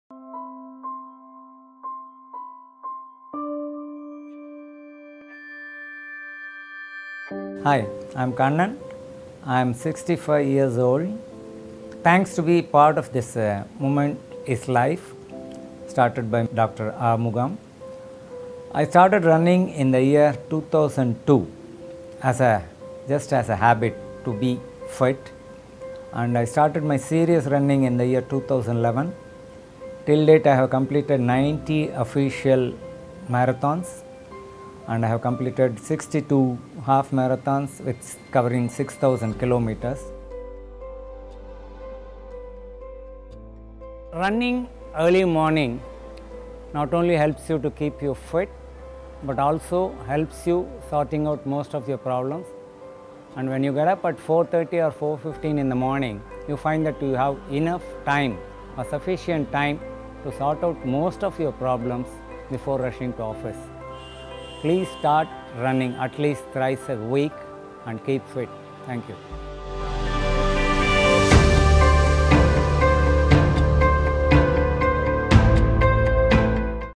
Mr. Kannan, a marathon runner shares his views on MovementisLife
Mr. Kannan Sankarambadi, a runner who destroyed the myth “running is a sport for the young.”

At the age of 57, he started his running career with a first marathon in 2011. Till date he has completed nearly 90 official running events and 62 half marathons, covering over 6,000 km. He challenges himself to improve each run. Running with youngsters motivates him to beat his personal best.

He believes it’s never too late to start anything and encourages everyone to start running.

We thank him for lending his support towards MovementisLife. Movement is Life campaign is initiated by Dr. S. Arumugam to spread awareness about arthritis.